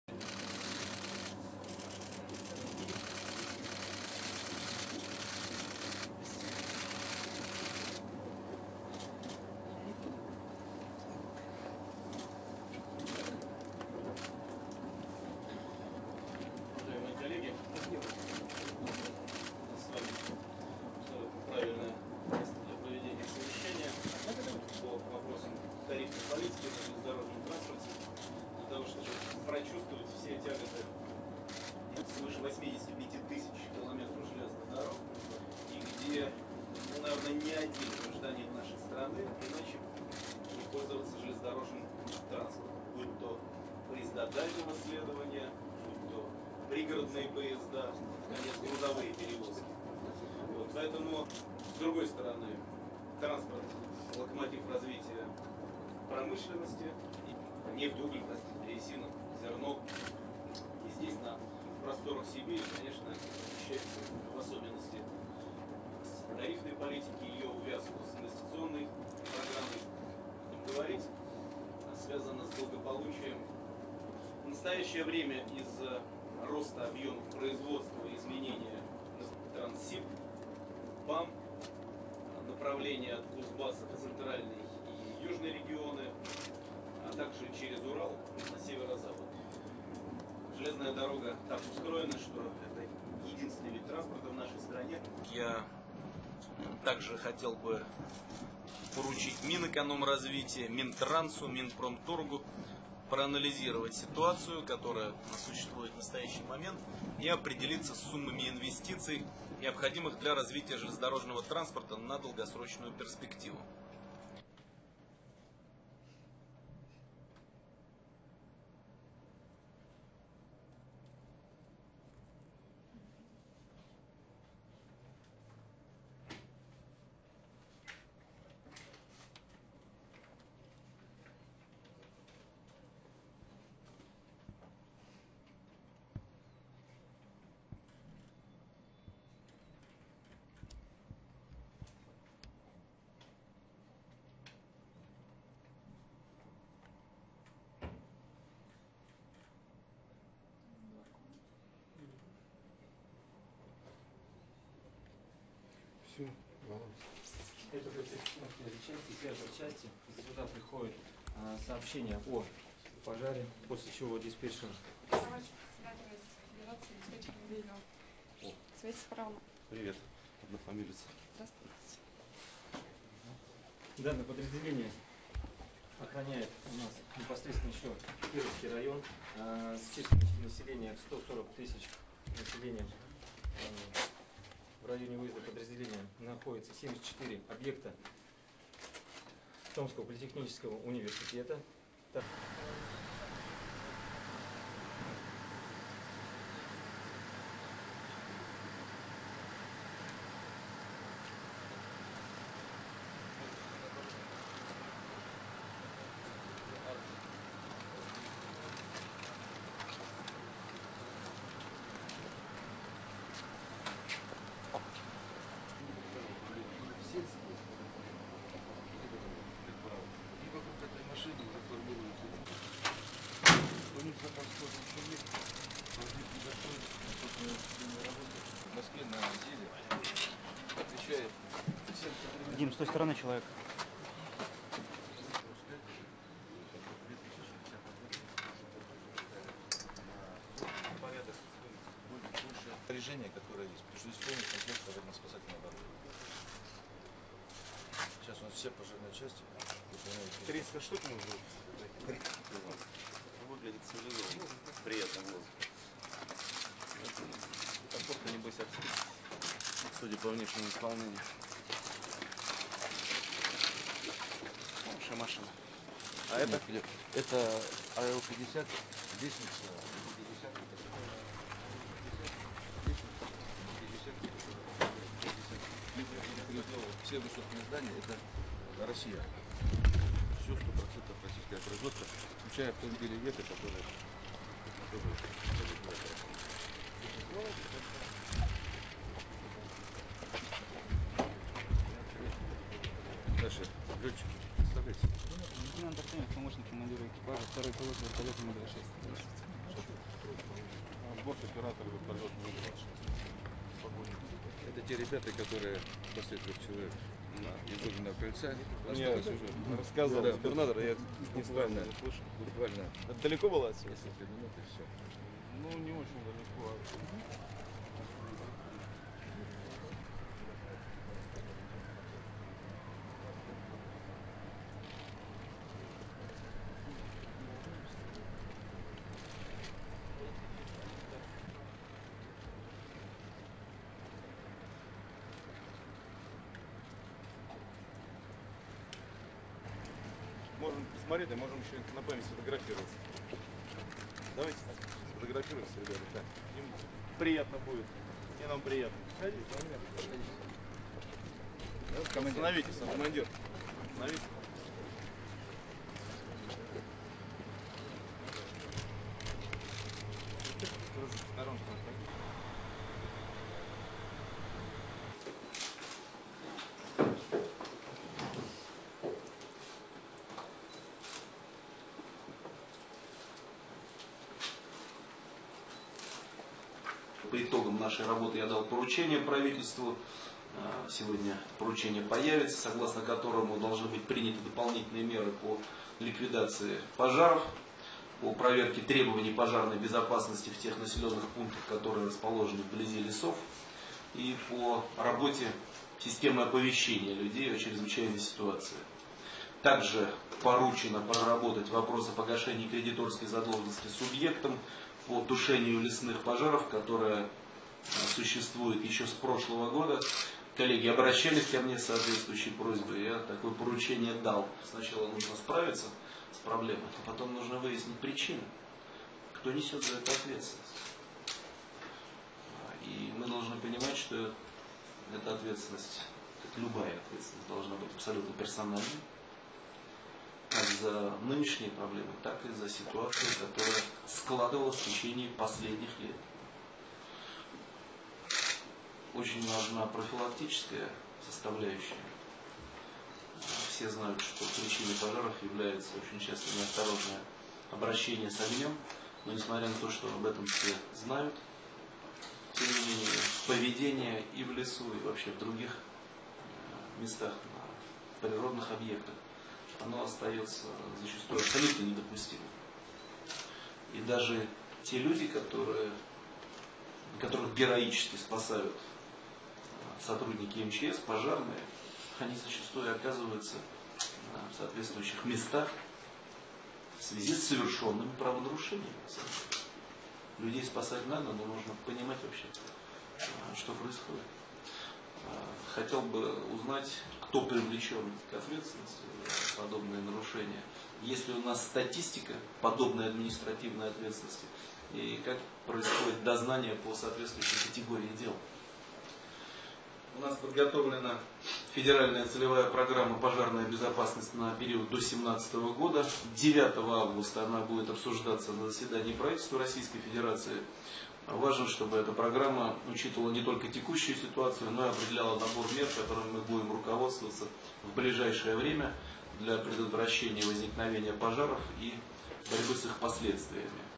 Aug 6, 2012 Russia_Medvedev discusses forest fire-fighting in Siberia
Russian Prime Minister Dmitry Medvedev arrived in the Siberian city of Tomsk on Monday, and met with regional officials to discuss ongoing efforts to tackle forest fires.

Around 180 wildfires are currently raging across Russia, according to the Emergency Ministry, with the largest in Siberian regions.

The premier inspected the equipment at a local fire station and met with staff.

Medvedev told officials that personal responsibility should be taken for failure to prevent fires.

"We need to tackle the problem first and then identify its causes and hold the responsible parties accountable on an individual basis, for both the current problems and the ones accumulated over the last few years. Prevention is also very important. We all know that quite often, forest fires are caused by being reckless with fire. But in spite of this awareness, people often act unacceptably in forests and other nature areas," he said.

On his way to Tomsk by train, the premier chaired a meeting on the development of the Russian railway network.